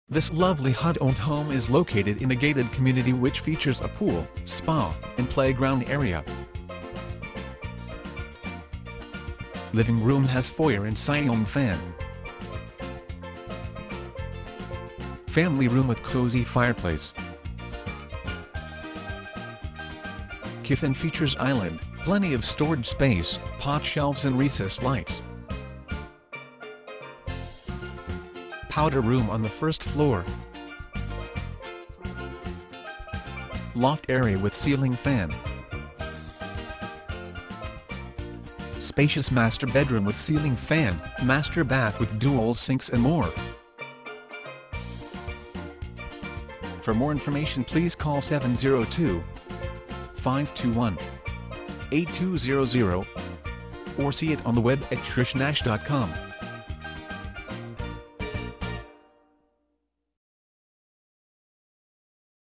724 Point Bluff St Henderson NV
WOWHUD Owned home!Gated community comes with this 3+bed (+is loft)2 1/2 bath homeComm has pool, spa and playground!Per FHA appraiser 2174 sq ftInsured Eligible for FHA 203KDisclosures: HOA Management fee of $90 Monthly: Real Property Management GroupAgents see attached MLS Addendum for all BLB Resources, Inc HUD homes & Overbidding & Escrow Repair Addendum Rev 5-21 and other attachmentsCall now to see!!

Video uploaded on Thu 12 Jul, 2012
real estate Henderson NV